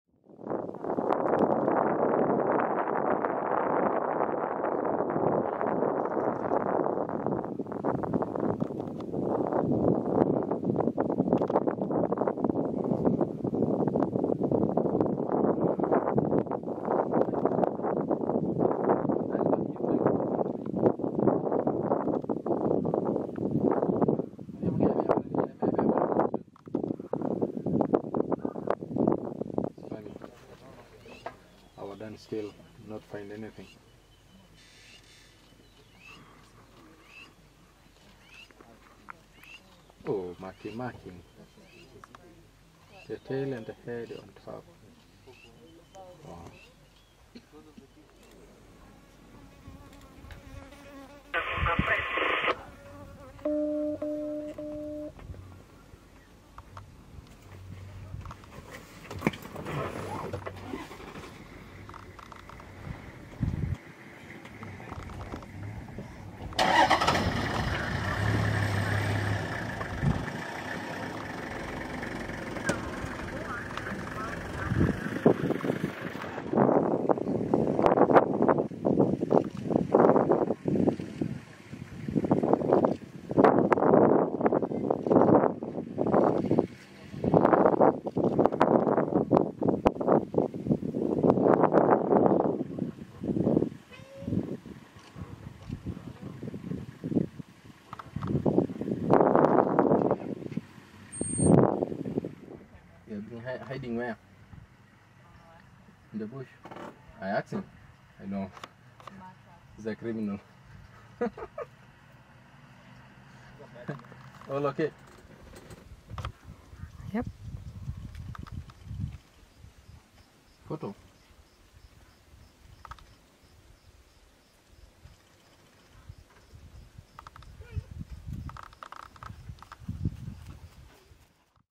🎬CHEETAHS MARK TERRITORY [4K]
The once famous NNE BORA cheetahs mark their territory.   Why do cheetahs mark their territory? A Cheetah will mark their territory by urinating or by cheek and chin rubbing. Saliva that is secreted contains the same chemical information about the animals, as does the urine. Cheetahs will mark territory so that they can better avoid one another.

 As you may know they were once known as the Tano Bora (Five male cheetah coalition) or The Fast Five, The Five Musketeers, Five Boys, and Five Brothers.  Now only three of the five remain and are now known as Tatu Bora.

Filmed in the Maasai Mara, Kenya
January 2023